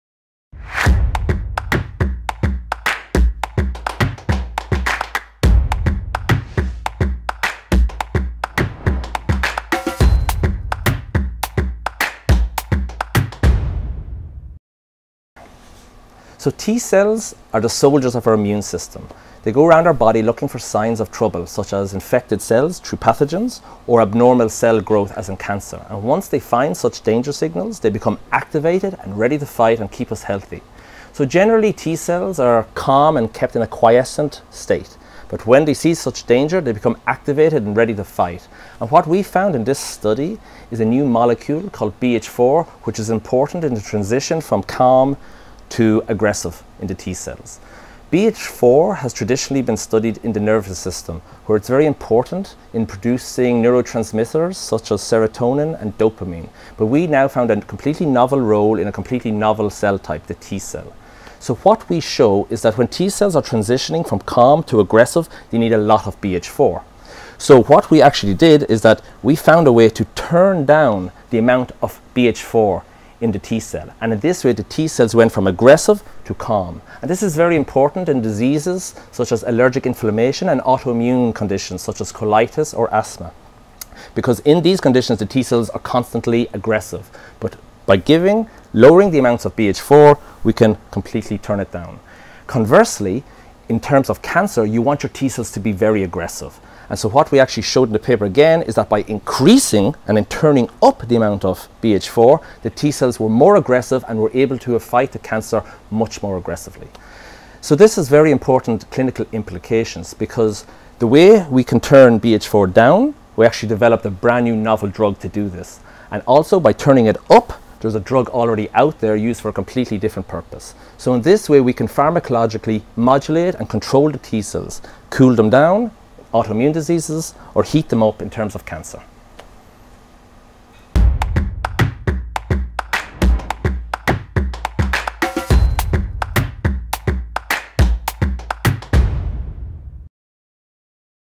Building block of "happiness hormone" is key to conducting immunity
Scientists at IMBA - Institute of Molecular Biotechnology at the Austrian Academy of Sciences - together with the Boston Children's Hospital at Harvard, demonstrate a completely new way of combating autoimmune diseases and cancer.
Shane Cronin, PostDoc at IMBA and first author of the study, talks about the main findings and why they matter.